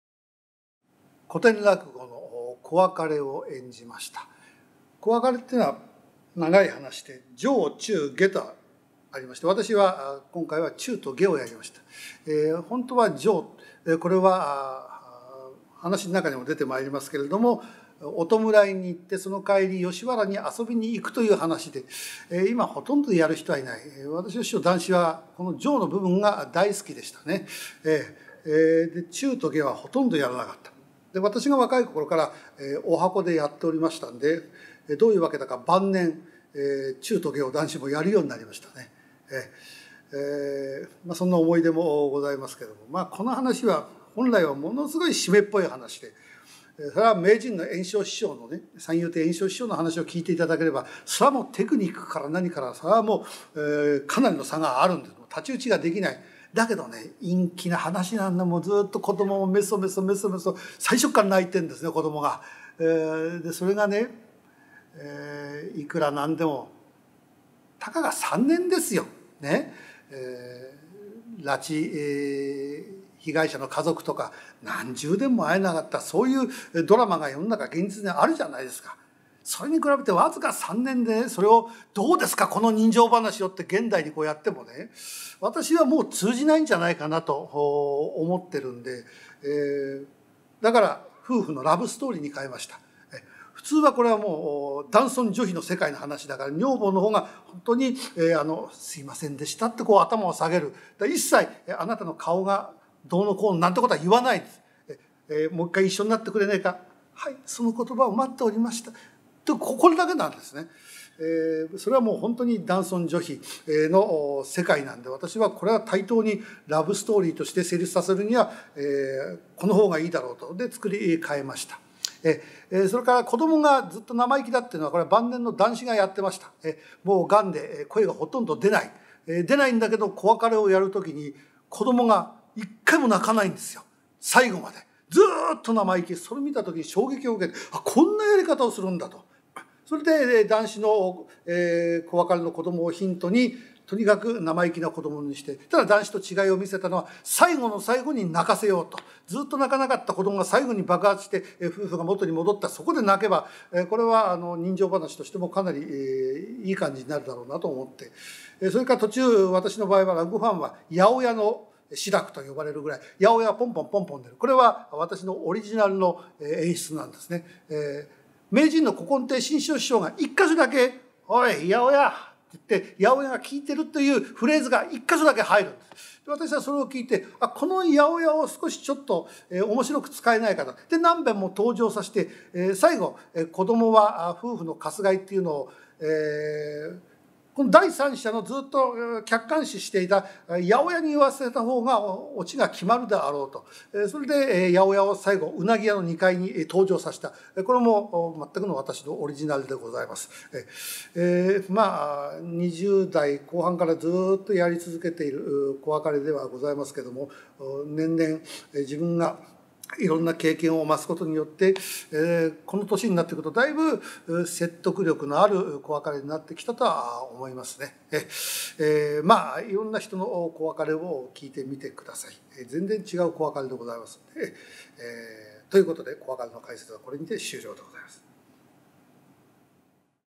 立川志らく「子別れ」解説
お聴き頂きました志らくの「子別れ」について、志らくこだわりの演出方法含め、本人がより分かりやすく解説しております。